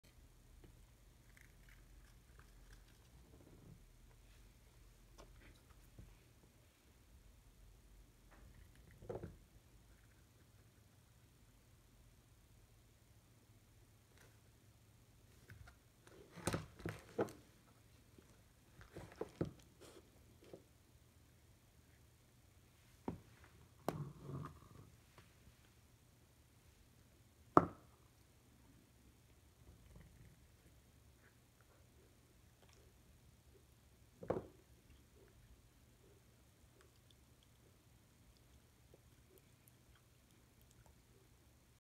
Curious 12 Week Old Golden Retriever Puppy
Hello my name is Theo, but you can call me Theodorable. I was born on 1/7/21, and I love cuddles, belly rubs, and squeaky toys.